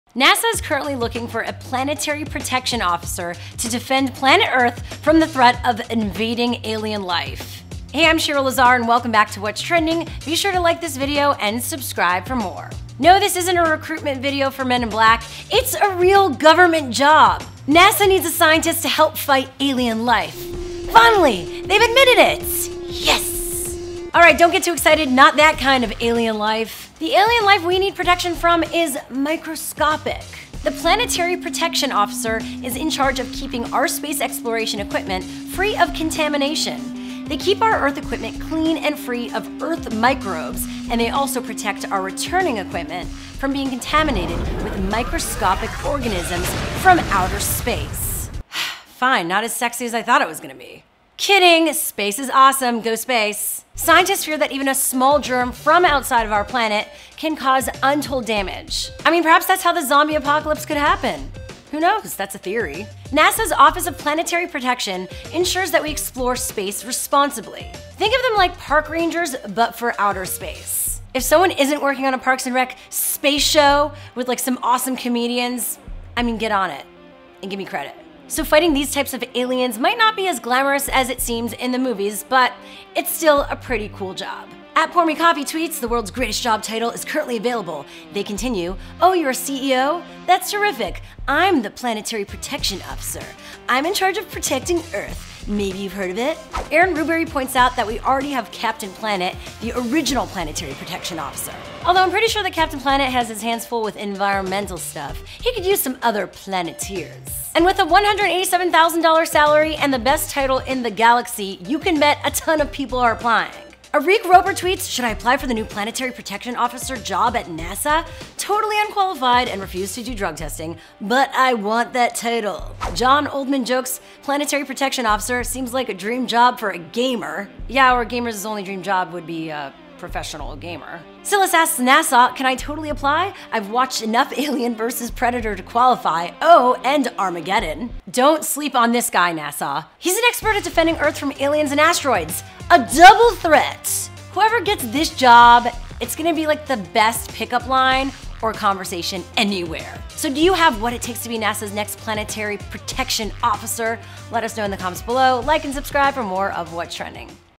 NASA Hiring "Planetary Protection Officer" To Protect Earth From Alien Life | What's Trending Now!
NASA Needs "Planetary Protection Officers" To Protect Earth From Alien Life

NASA is currently looking for a Planetary Protection Officer to defend planet Earth from the threat of invading alien life.